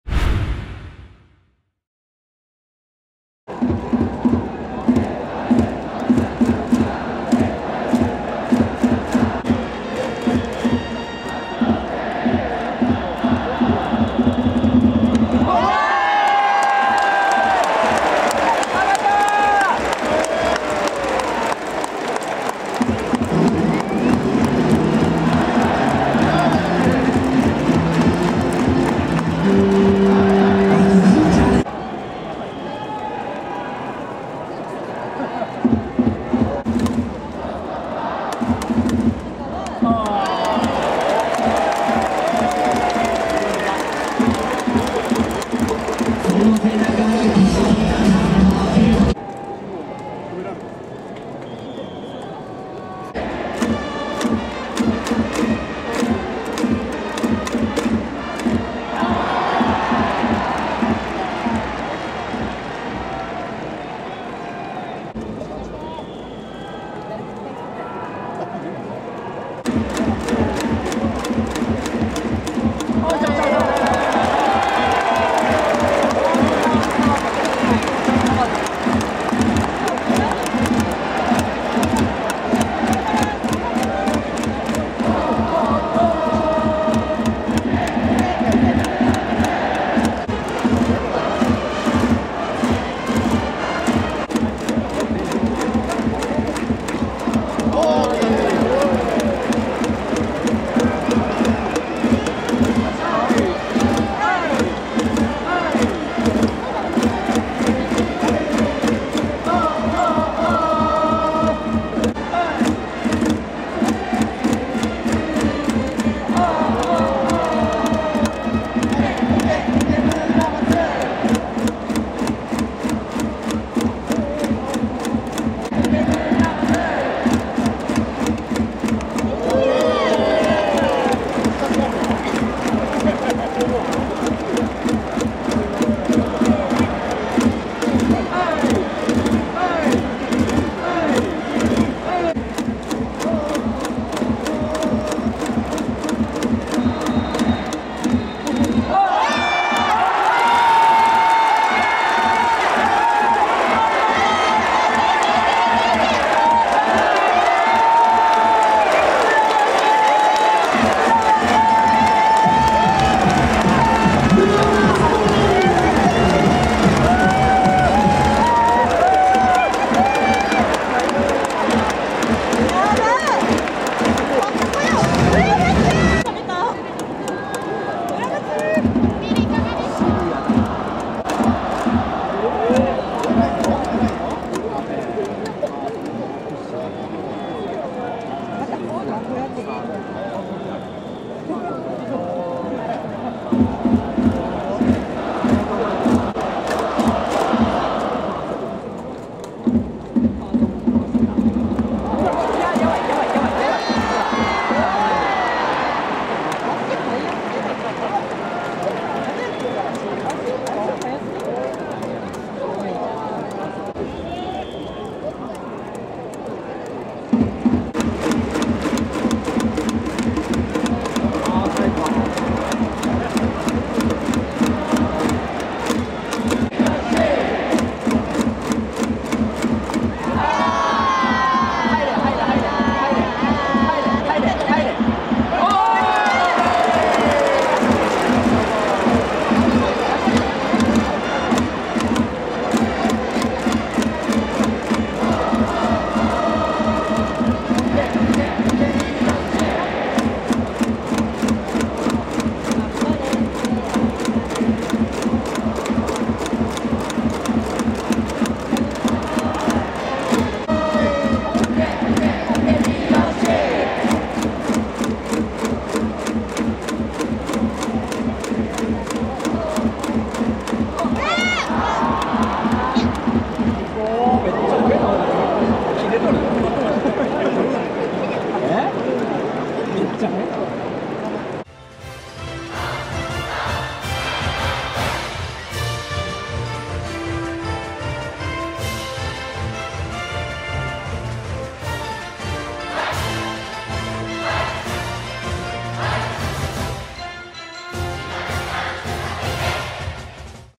巨人メンデス投手, 自身の不甲斐なさに怒りの一塁送球にどよめく場内！観客から「怒ってる？」の声も！岡本和真選手はしっかり捕球もベンチで見つめる阿部監督！2回裏の攻防撃
巨人vs中日 2回裏の攻防撃

メンデス投手, 自身の不甲斐なさに怒りの一塁送球にどよめく場内！
岡本和真選手はしっかり捕球もベンチで見つめる阿部監督！